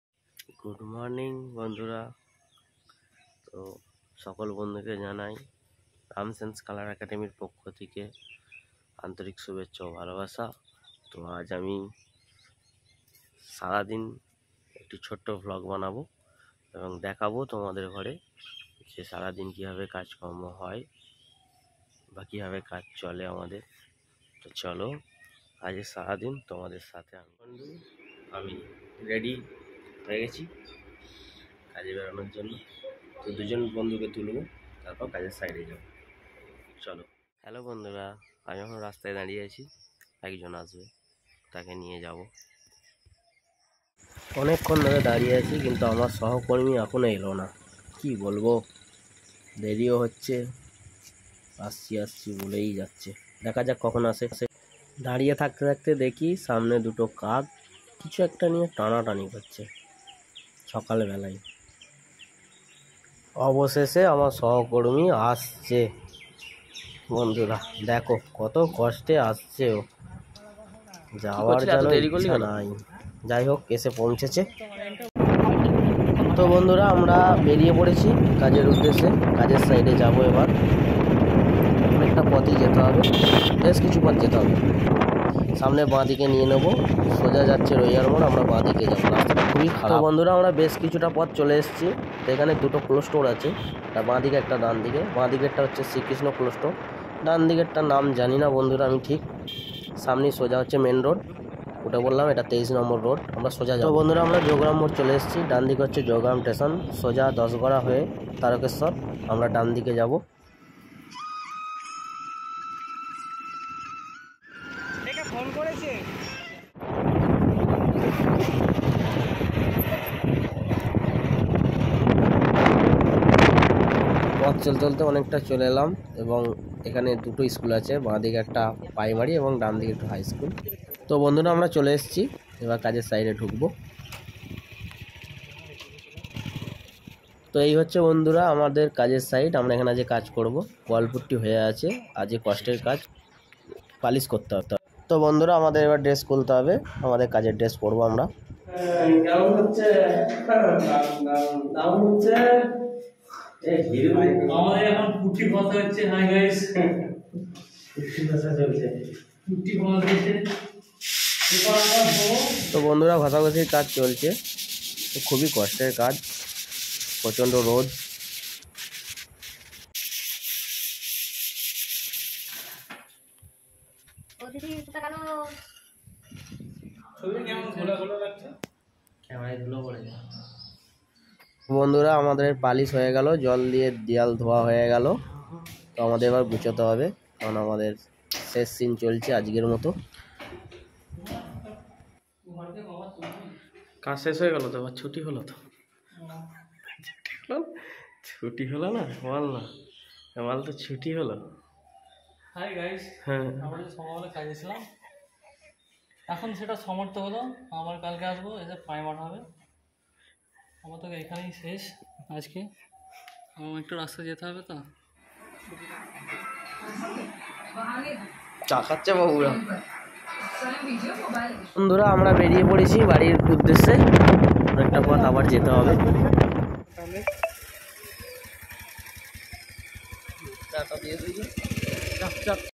আজ আমরা কি ভাবে কাজ করি ও সবকিছু একটা ছোট্ট vlog ভিডিও করে দেকাবো তোমাদের বন্ধুরা,
Ram sen colours academy new blog video